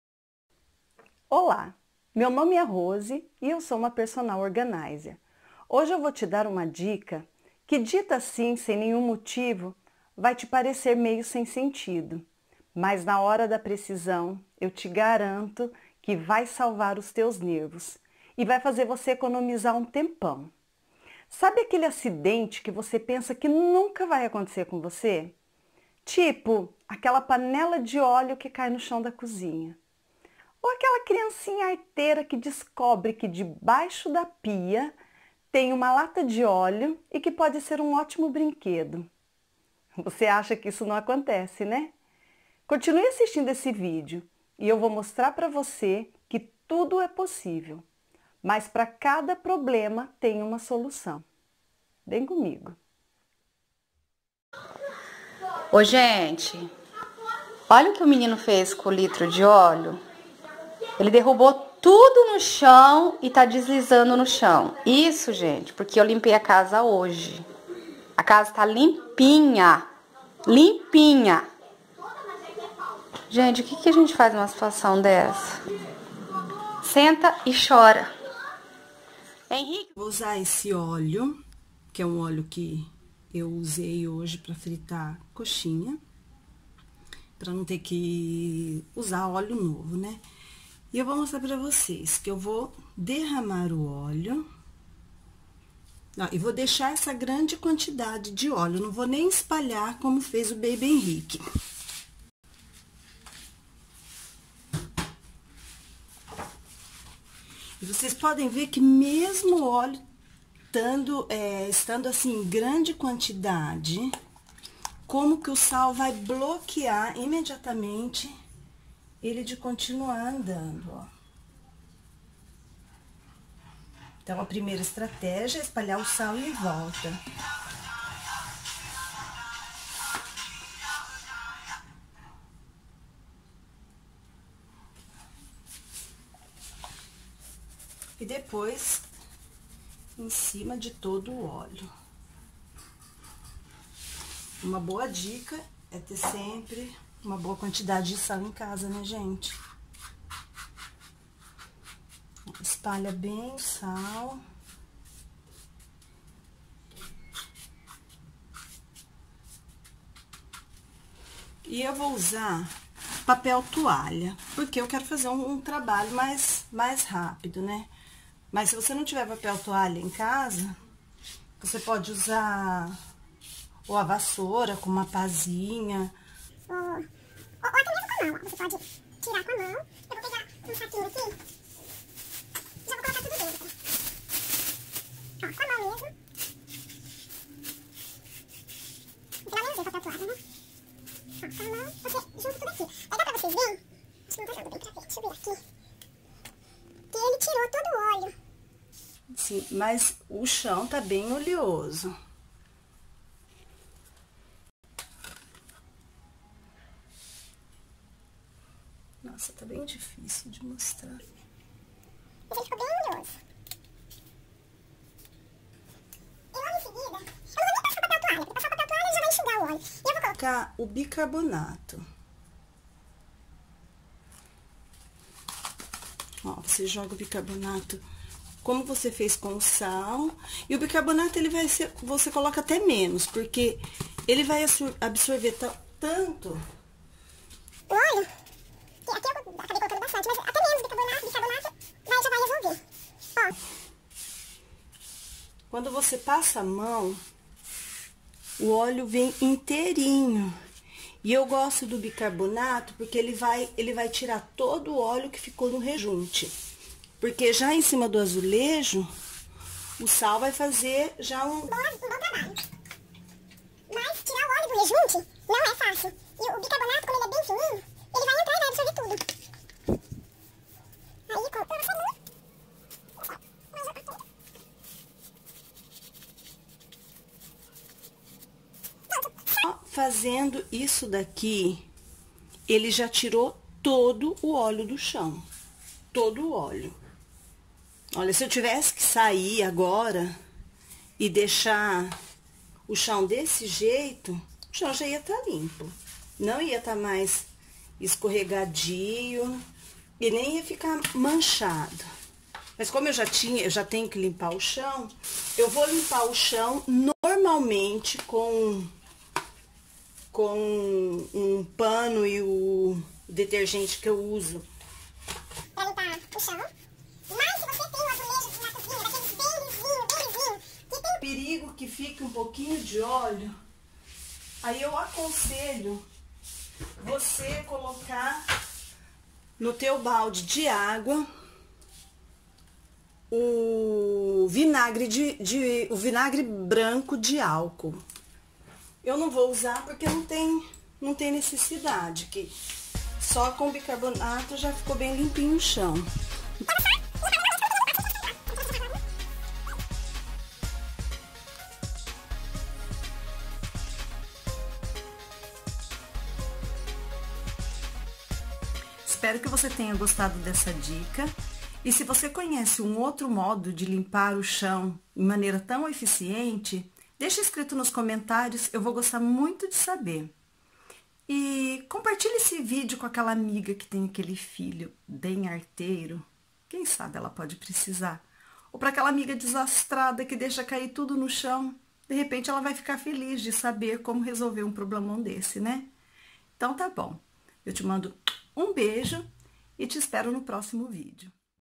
Como limpar de forma rápida e eficiente óleo no chão da cozinha!
Com dois ou três ingredientes que normalmente já temos em casa eu vou te mostrar nesse video como é fácil limpar sem stress aquele óleo que por um acidente (ou não) caiu no chão da cozinha. Assista até o final e me conta se isso já aconteceu com você. Com essa dica em poucos minutos eu te garanto que o teu piso vai ficar limpinho e sem nenhum vestígio de óleo.

Me sigam também no instagram: @rose_viacava_